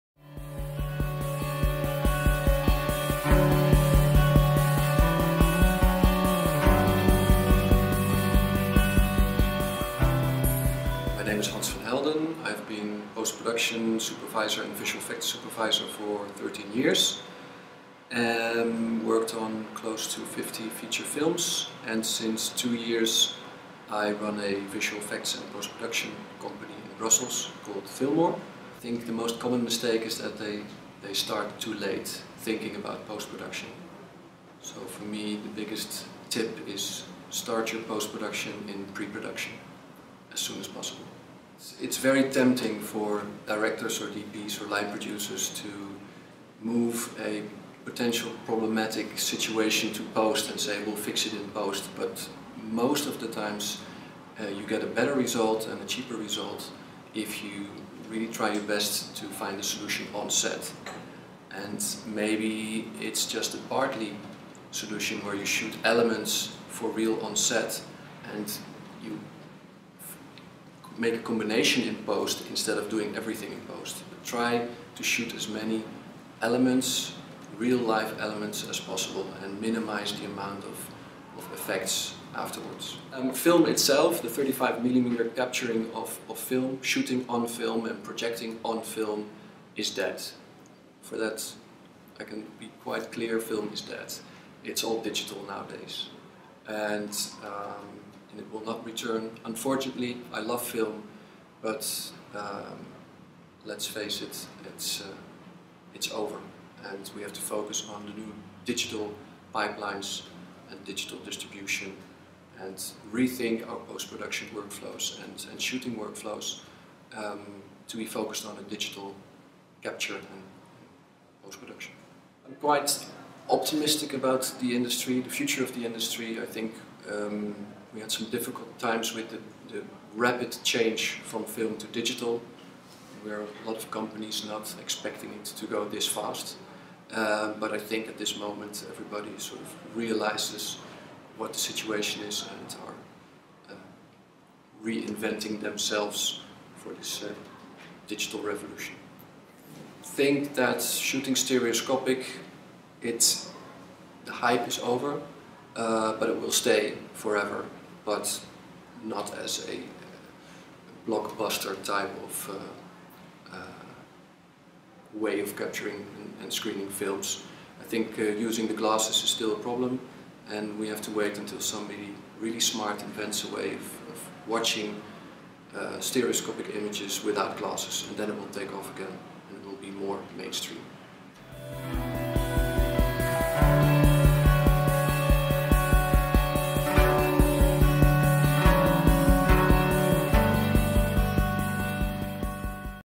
Hans van Helden, Filmmore
"You get a better result and a cheaper result if you really try your best to find the solution on set"
Hans van Helden, VFX Supervisor, VFX Producer, Filmmore (MEGA Plus expert)